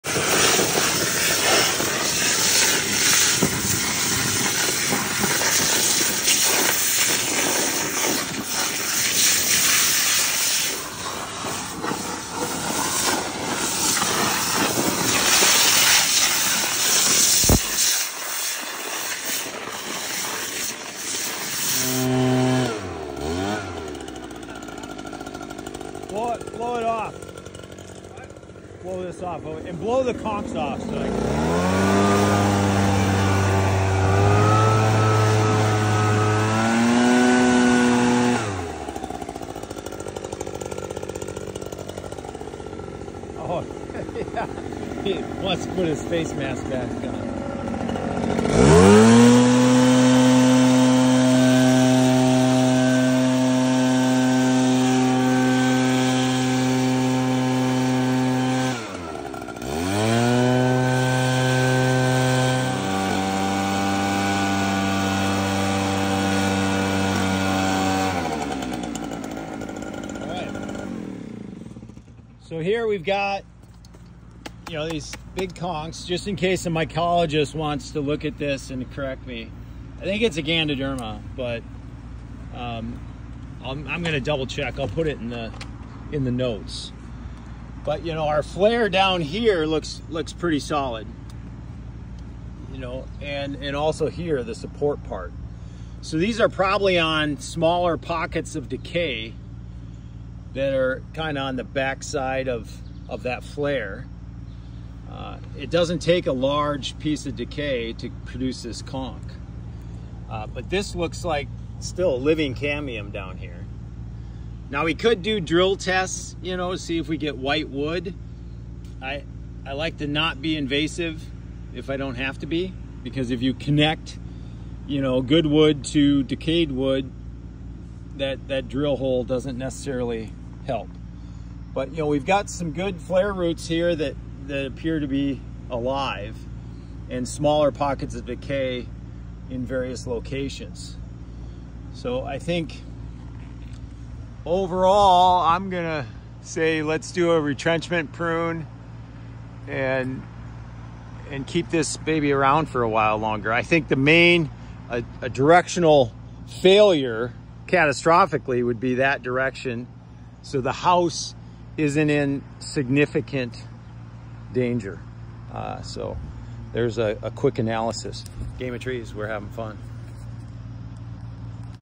Root Crown Investigation to Save a Tree
I believe the mushroom conks on this tree are Gandoderma sessile. Ganoderma sessile is a white rot fungus-it breaks down lignin and cellulose in wood, leaving it fibrous and pale.  The decaying wood tends to be more flexible vs brittle like a brown rot would be.